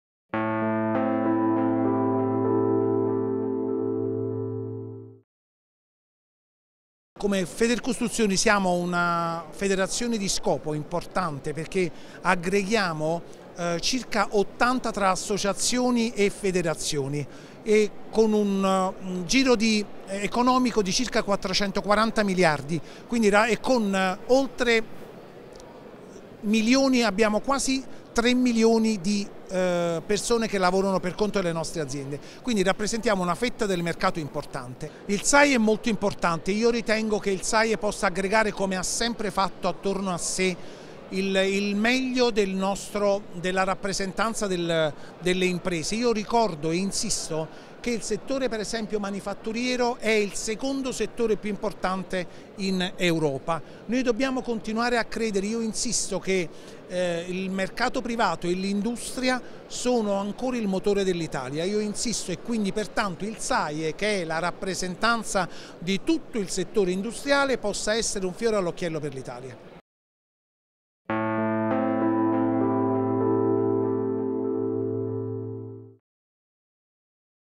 Convegno ANCE Roma: SAIE 50° intervista Rudy Girardi
Rudy Girardi, Presidente FEDERCOSTRUZIONI, intervistato il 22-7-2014, in occasione dell’assemblea annuale dell’ANCE - ASSOCIAZIONE NAZIONALE COSTRUTTORI EDILI, sull'importanza di Federcostruzioni nel panorama dell'edilizia italiana, oltre che sul ruolo del SAIE 50° Built Environment Exhibition, che si terrà a BolognaFiere dal 22 al 25 ottobre 2014 - video by Publivideo 2